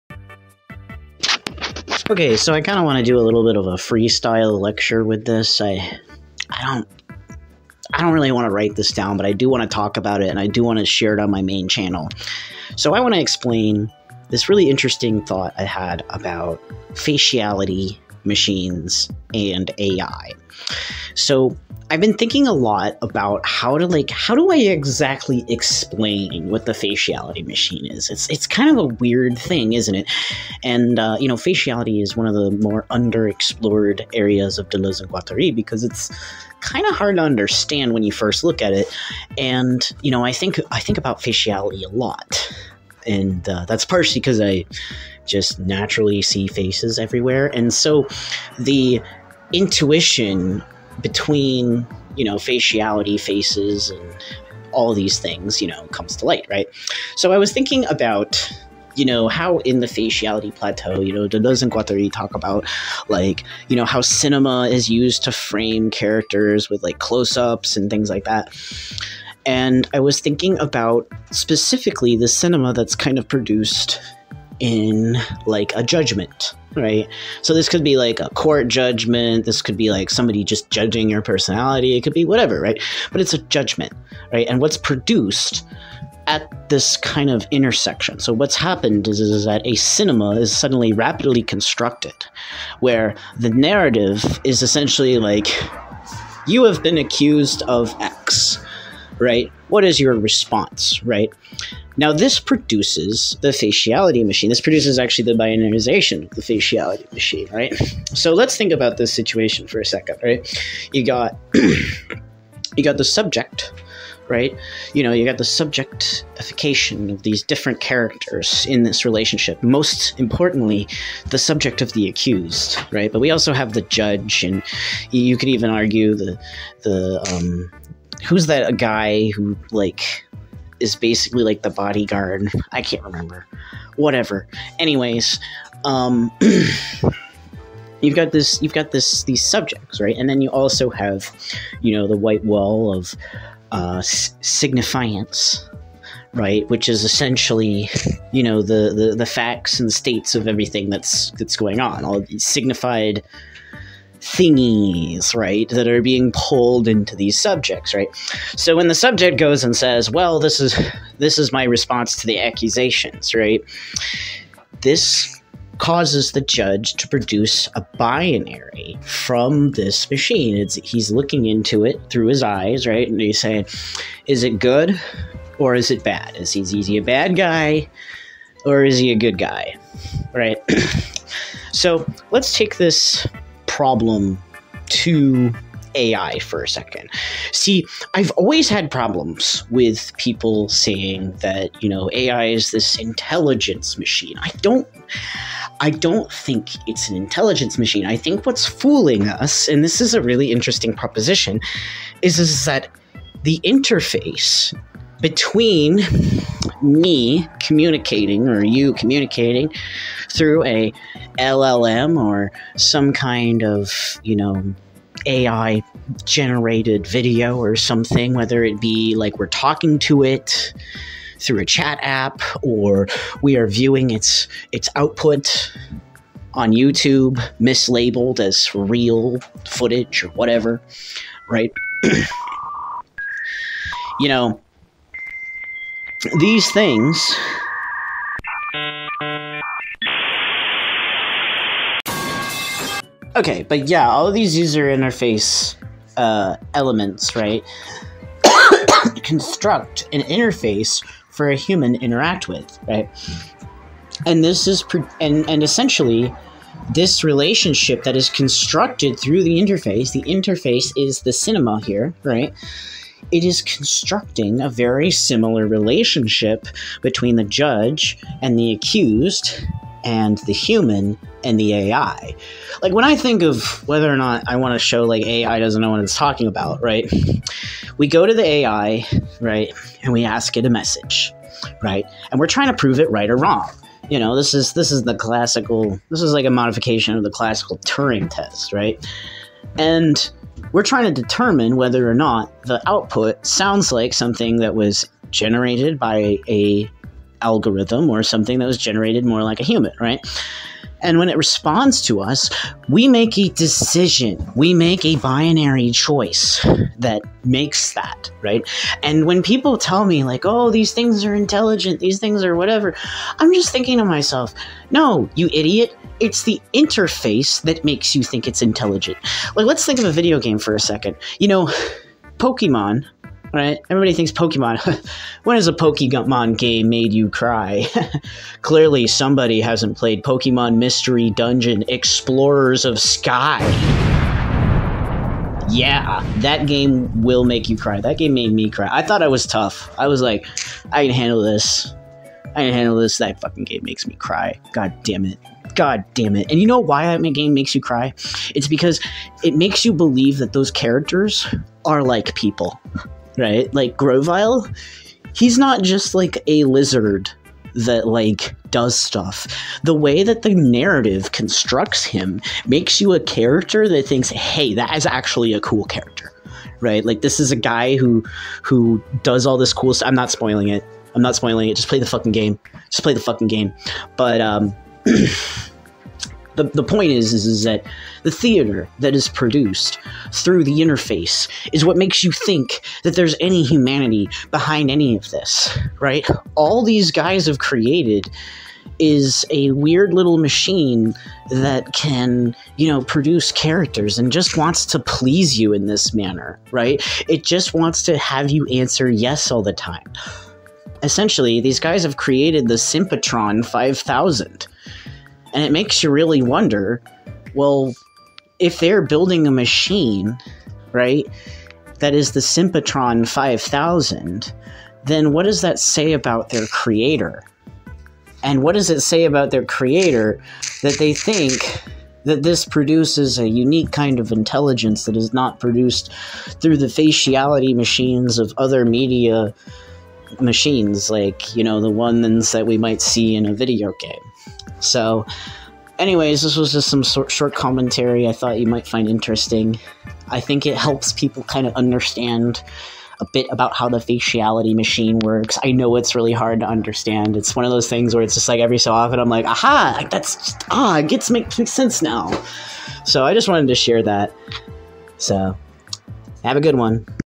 how i know nobody has proven AI is intelligent
a quick unscripted lecture on how the faciality machine and its relationship to the turing test demonstrates that we cannot know anything valuable about the nature of "intelligence" of LLMs outside of them being pleasuring machines